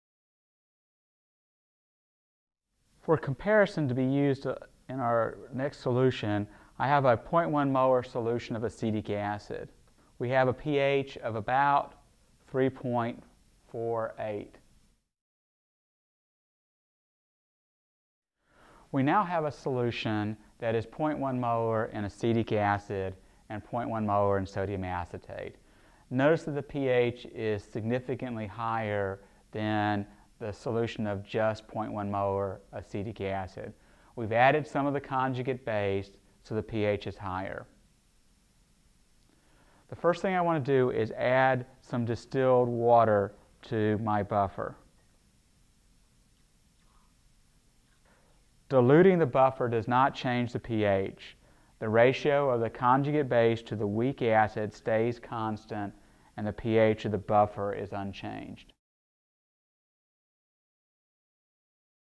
Dilution of Buffer - Buffer Activity Lab Part 2
Part of NCSSM CORE collection: This video shows the addition of water to a 0.10 M CH3COOH/ CH3COONa buffer as part of the buffer activity lab.